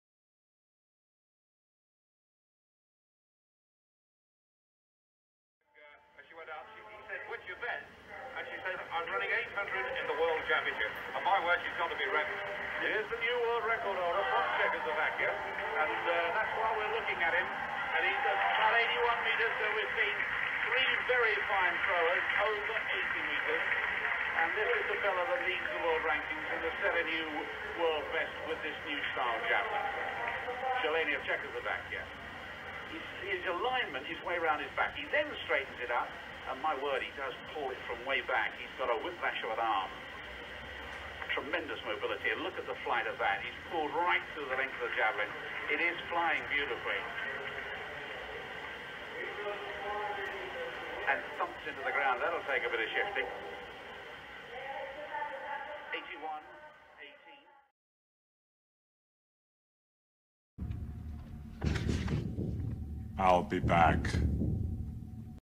Jan Zelezny (Czech Republic) javelin 81.18 meters Praha 1987-06-27 European Cup Final A
on VHS, VHS-c, BETAMAX & VCC I digitize for free.
 Greets: Gerlof Holkema